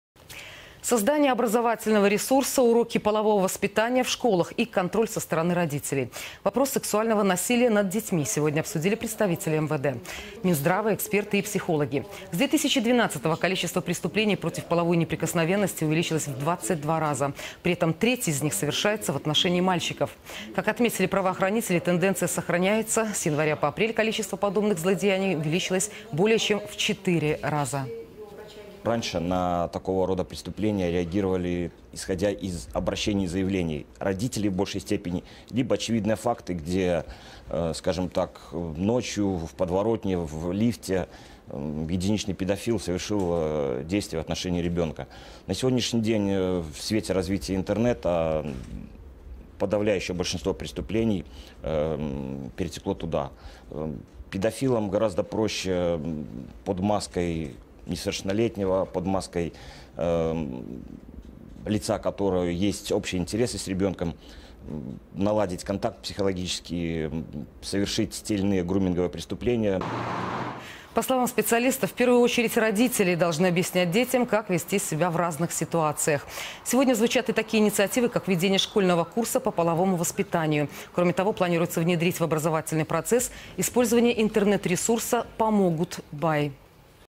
С 2012-го количество преступлений против половой неприкосновенности увеличилось в 22 раза
При этом треть из них совершается в отношении мальчиков. Вопрос сексуального насилия над детьми сегодня обсудили представители МВД, Минздрава, эксперты и психологи.